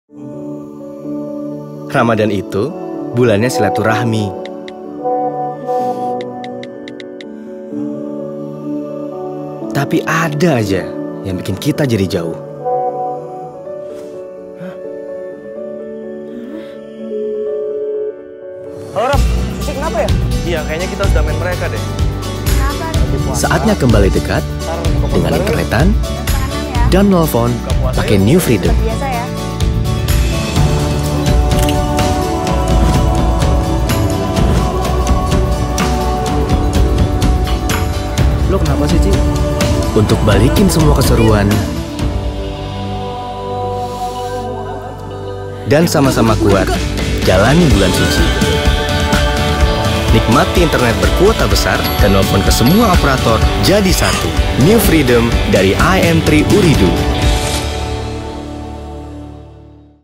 Ramadan Makin Kuat dengan Paket New Freedom dari IM3 Ooredoo
Sambut bulan suci Ramadan dengan tetap terhubung ke semua orang tercinta. IM3 Ooredoo siap mendukung silaturahmi kamu dengan Paket New Freedom untuk internetan tanpa ragu di jaringan 4G Plus Kuat! RamadanMakinKuat dengan kuota besar hingga 60GB dan gratis nelpon ke semua operator. Ayo, aktifkan di *123# atau myIM3 dan makin nyaman internetan di bulan Ramadan! Belum pakai IM3 Ooredoo? Beli sim card nya sekarang!